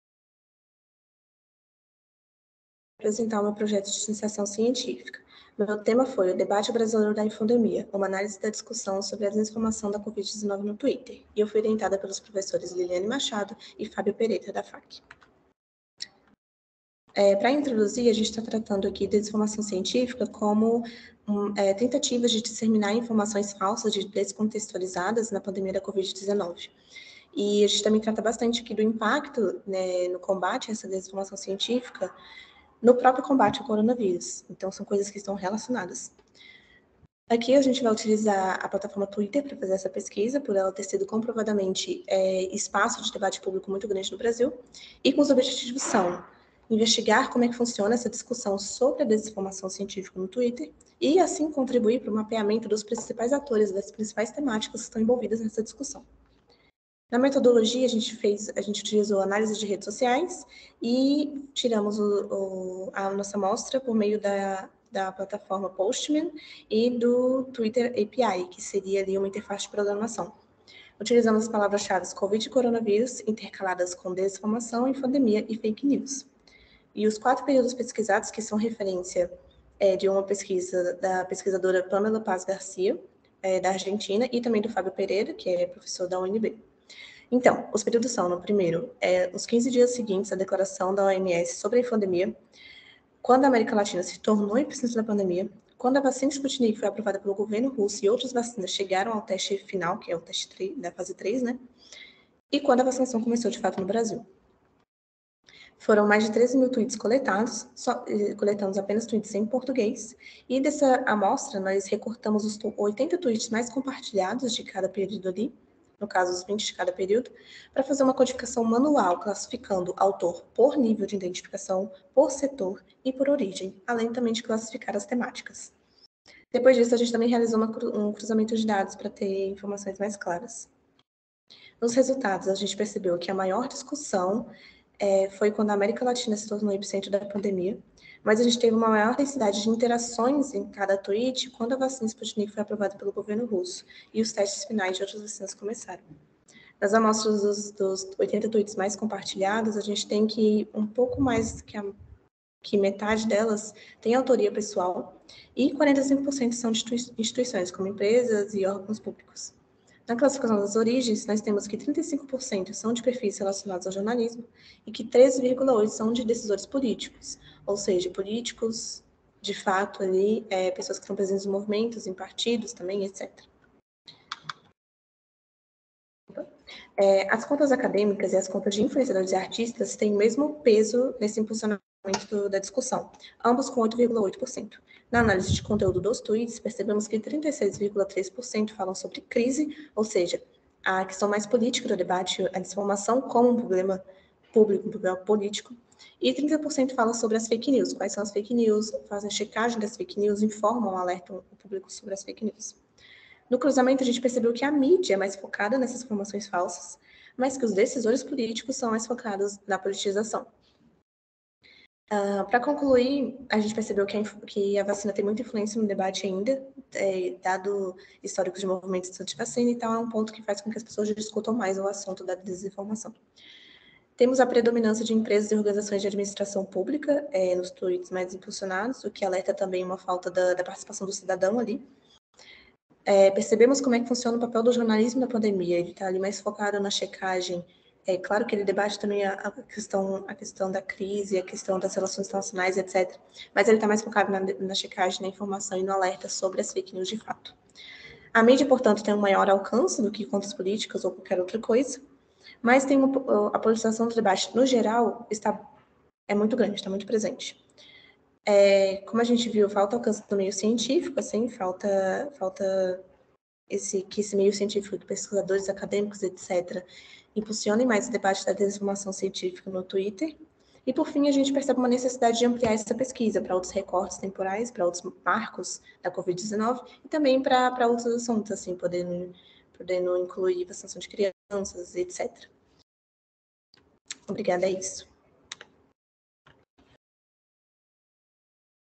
Desinformação científica no Twitter: uma análise de tuítes envolvidos na disseminação de notícias
Desinformação científica no Twitter: uma análise de tuítes envolvidos na disseminação de notícias falsas em 2021
Grande área: Artes e Humanidades
Aluno: BRENDA DA CONCEIÇÃO SILVA
Orientador: LILIANE MARIA MACEDO MACHADO